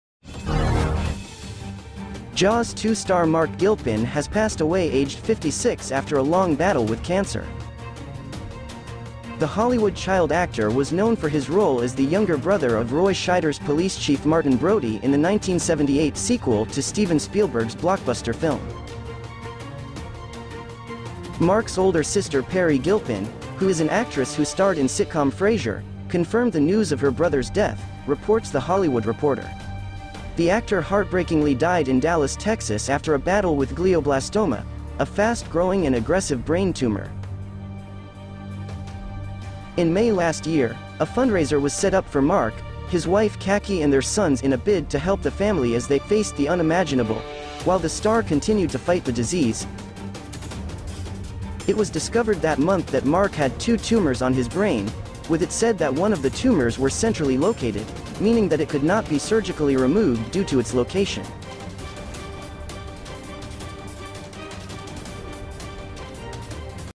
Marc Gilpin Dies at 56 - Young Actor on ‘Jaws 2' - Celebrity News
Marc Gilpin, who played the younger son of Roy Scheider’s Police Chief Martin Brody in Jaws 2, has died. He was 56.